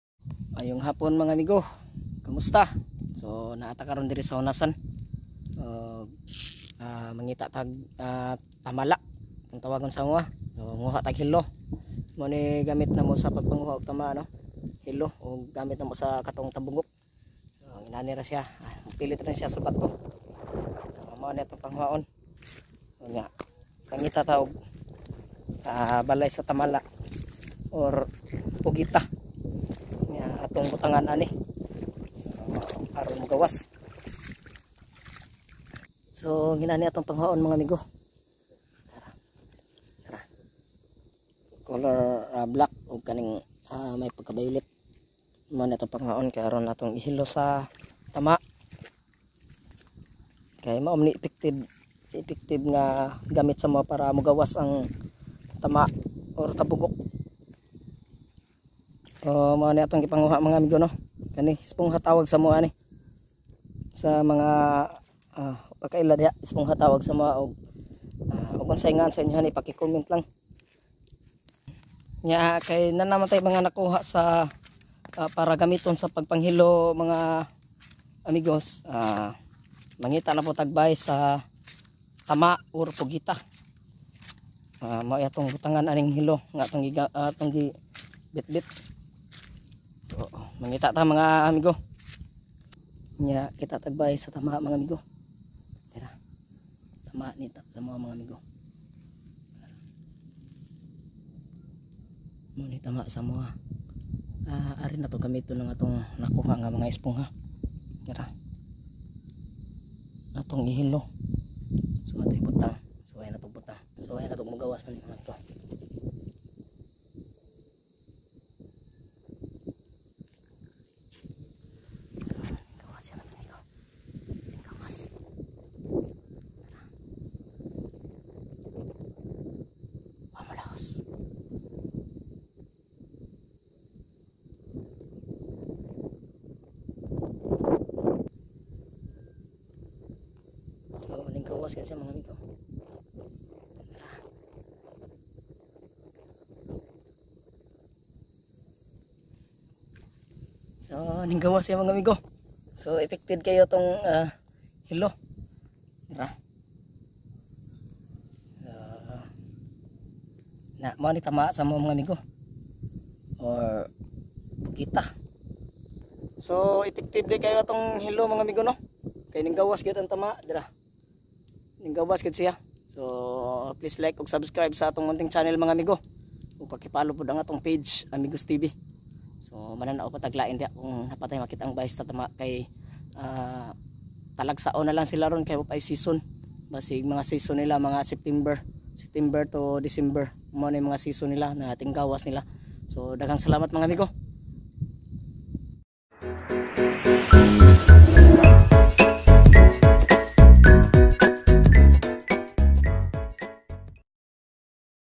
Spongha mabisang pampalabas ng pugita🤔
Wazz up mga idol for to this video kay manguha tag tamala o pugita gamit ang spongha kung tawagon sa amoa so tara mga mego mag uban ta..